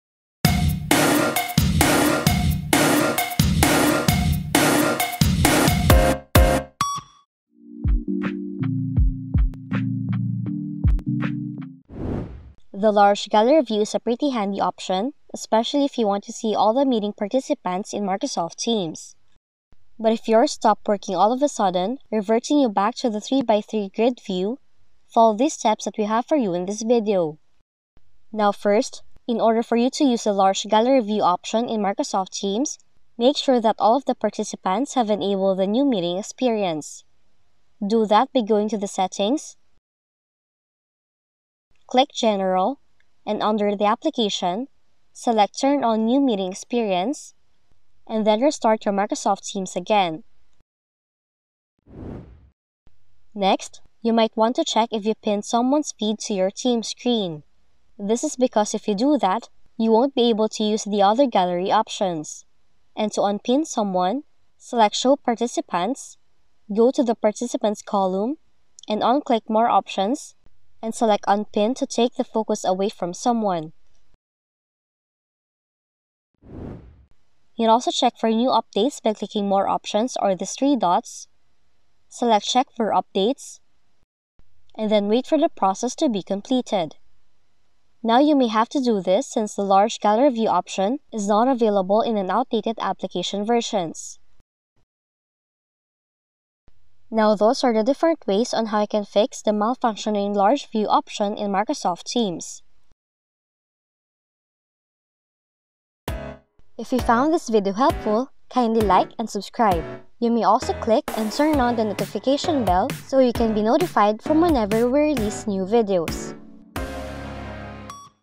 [FIX] Large Gallery View Not Working in Microsoft Teams
Large Gallery view is a nice option if you want to be able to see everyone in a Teams meeting. However, some users have reported Large Gallery view not working properly. If it notice it isn't working right, try checking these options outlined in the video.

Make sure all participants have enabled the "New Meeting Experience"

Make sure you didn't accidentally pin someone's video to your screen

Update Teams to the latest version.